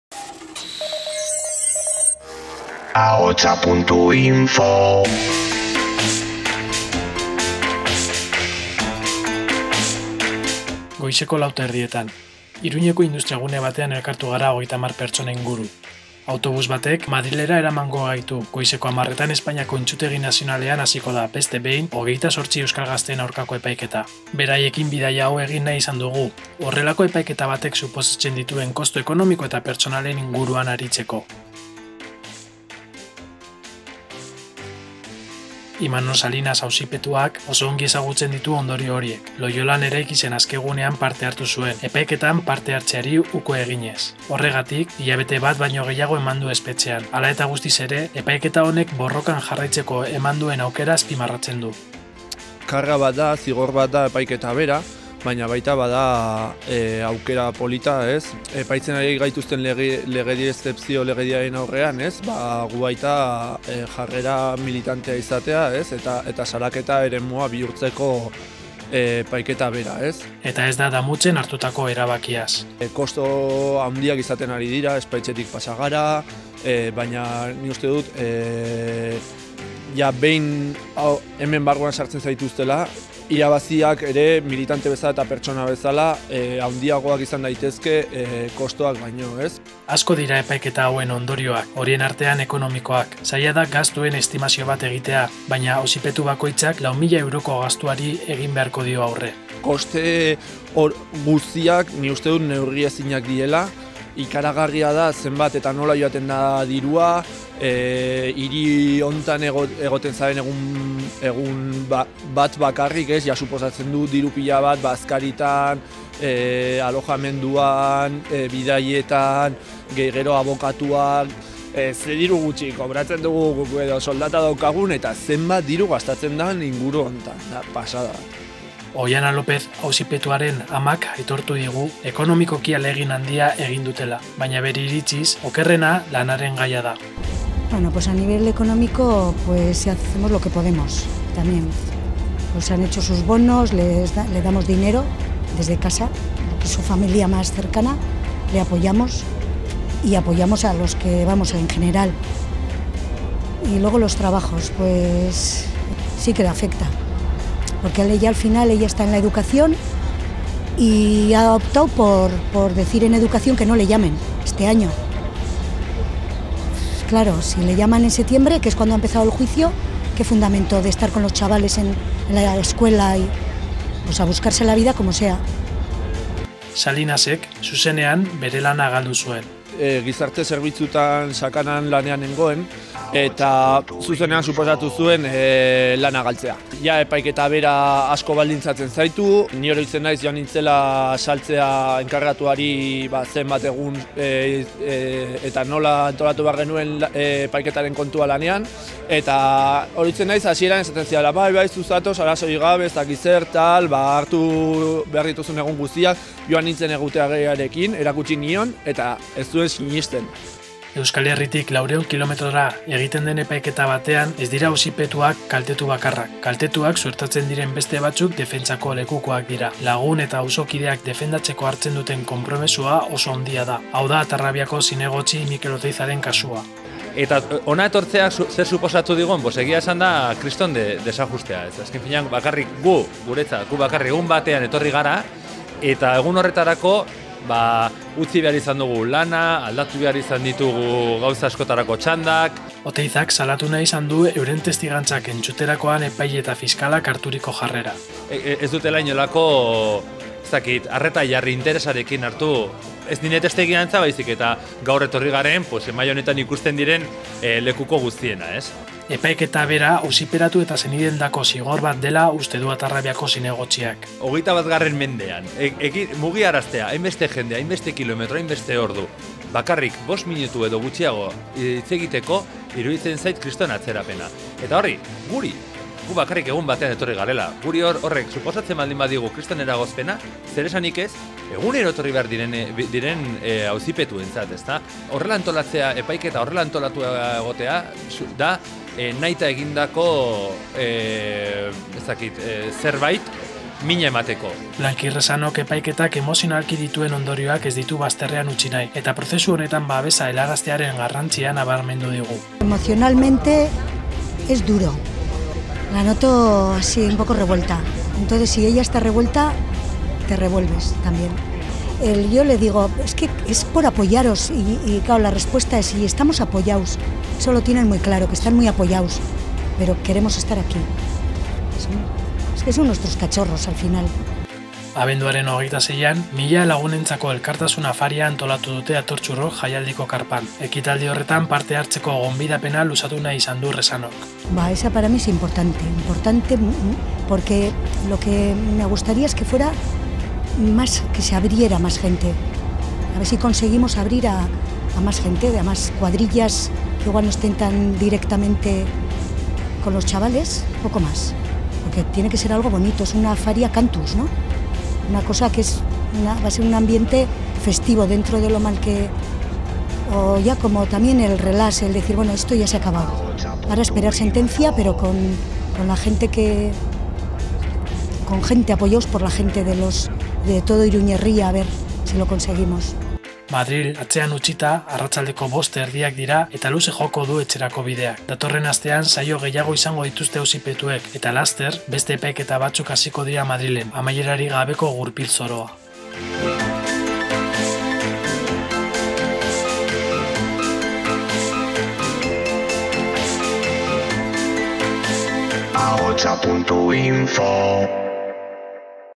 28akEreLibre Epaiketa politikoak, "mina egiteko" prozesuak
Epainiako Entzutegi Nazionalean epaitzen ari diren 28 euskal gazteekin bidaia hau egin nahi izan dugu, horrelako epaiketa batek suposatzen dituen kosto ekonomiko eta pertsonalen inguruan aritzeko.